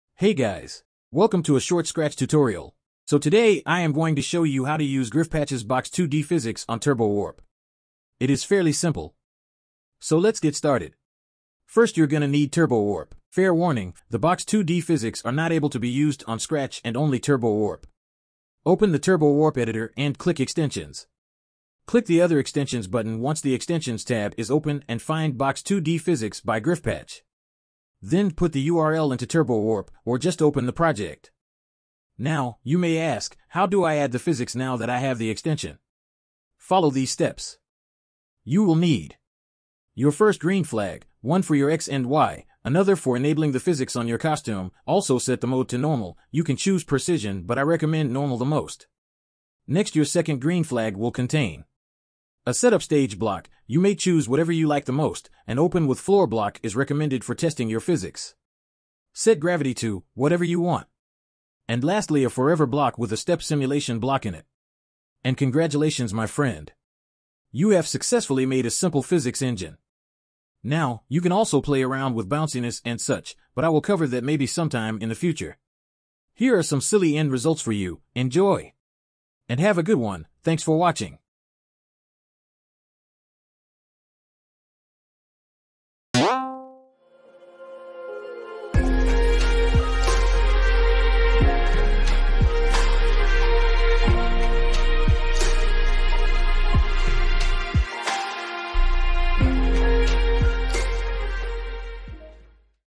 Physics Tutorial(Box2D) || TurboWarp/Scratch.
First Episode of the Scratch Tutorial Series

I hope you enjoy, this is my first tutorial and it is kinda going to be low quality, but thankfully i will explain all the way, so no worries

And go ahead, share feedback in the comments on what i should improve, i appreciate them :P